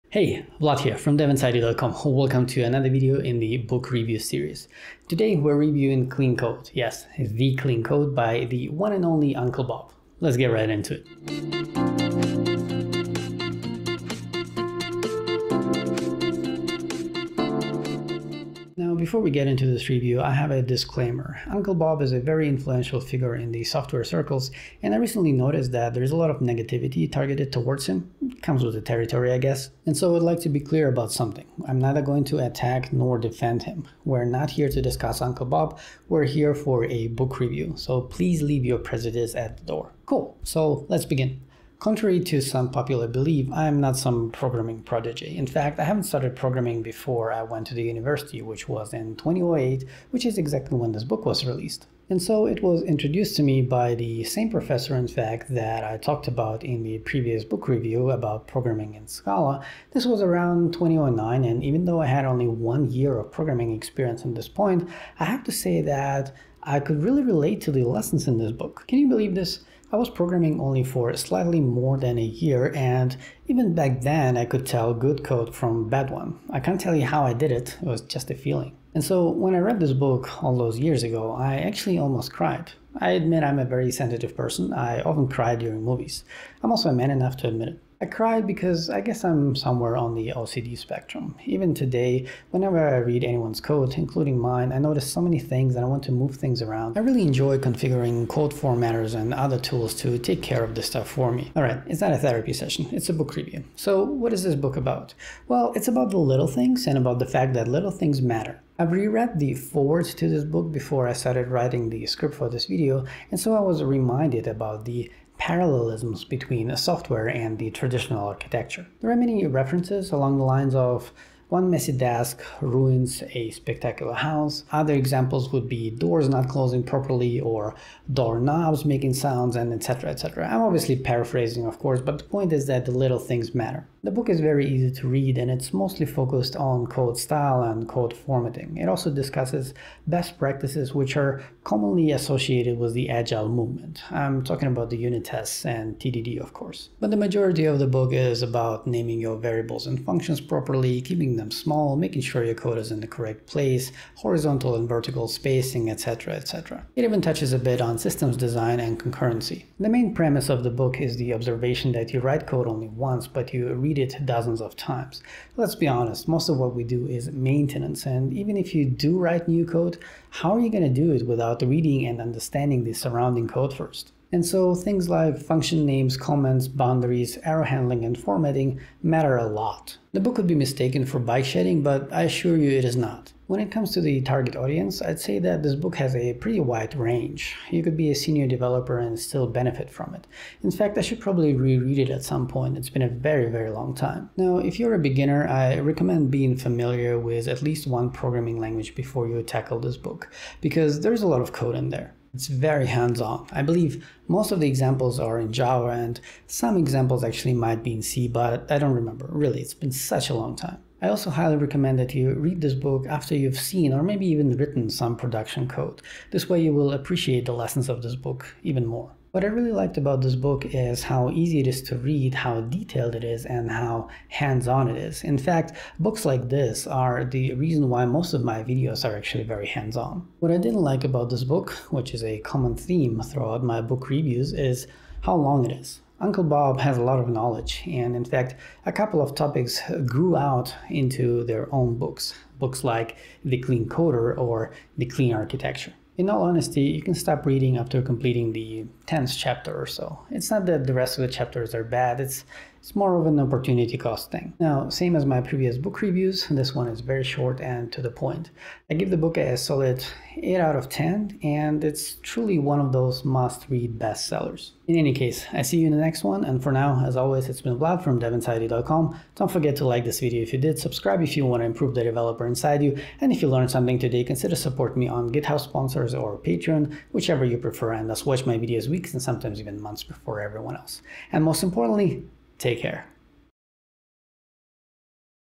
"Clean Code" Review
This is a book review of the one and only Clean Code by Robert C. Martin aka Uncle Bob.

Table of Contents:
00:00 Intro
00:41 Meta Info
01:44 Content
03:03 Target Audience
03:39 Conclusion

Cheers and Happy Coding!